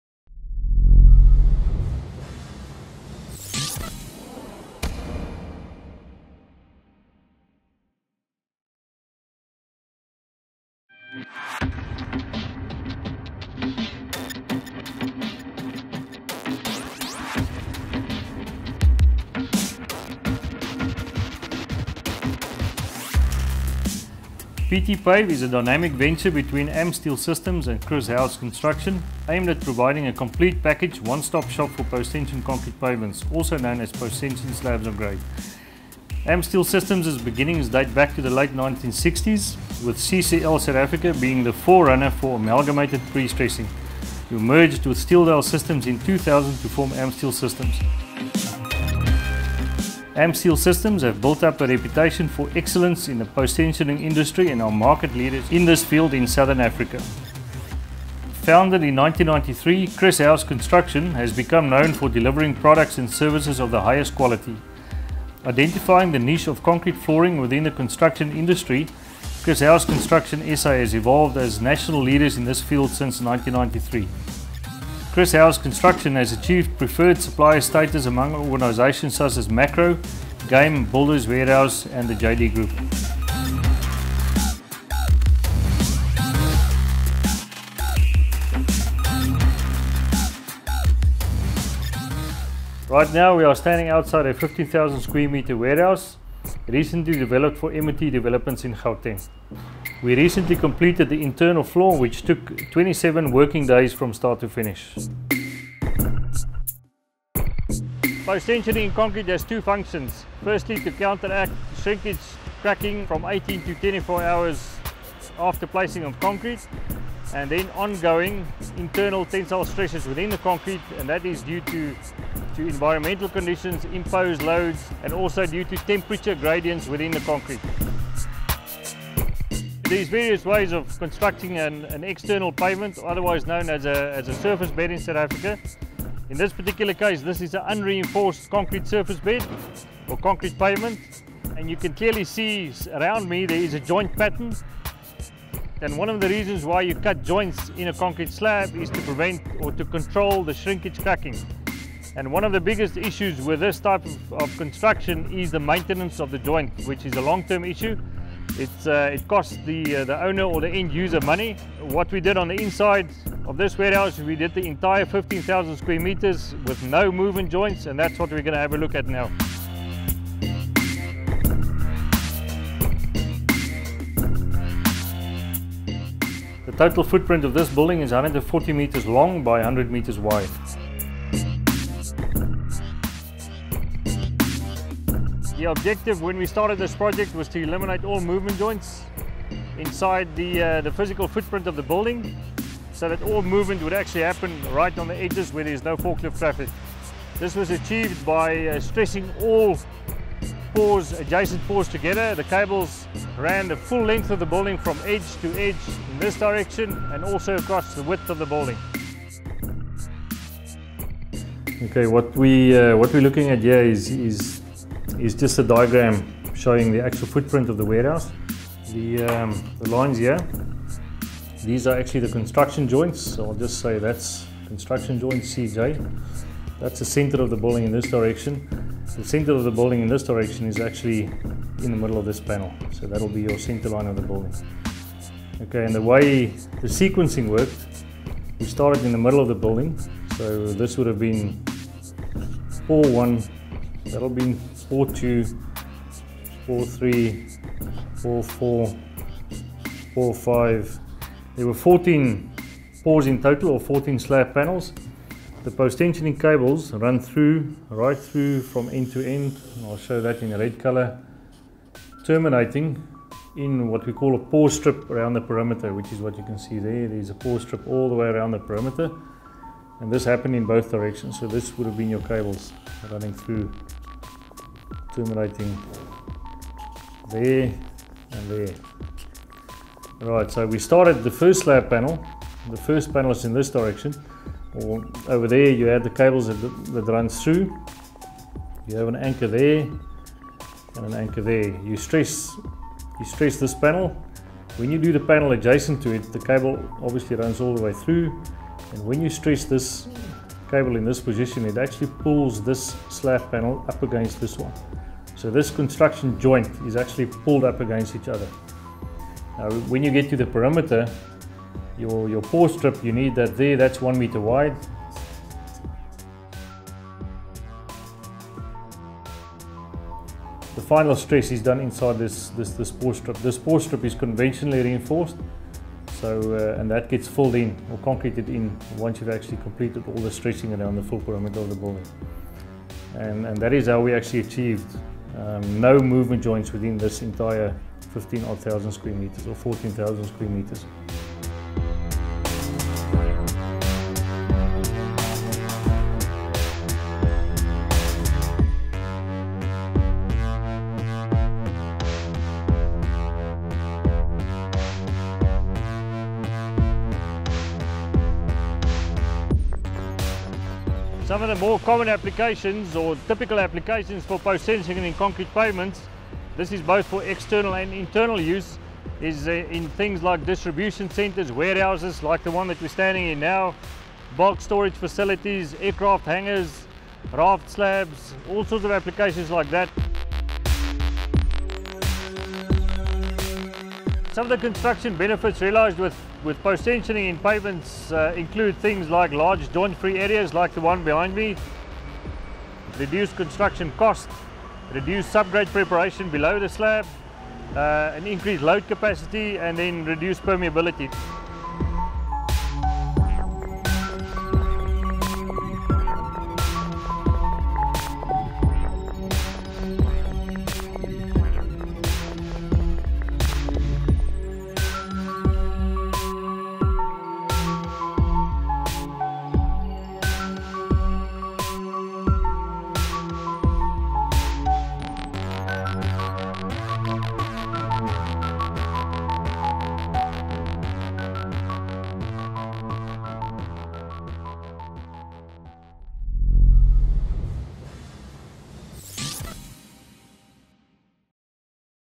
Exclusive timelapse video showcasing post-tensioning in concrete pavements (construction)
Amsteele showcases an optimum method of constructing internal and external industrial concrete pavements. This system provides a joint-free and crack-free area with greatly improved durability. It is not only more economical but also a more effective method compared to conventional or fibre-reinforced concrete pavements.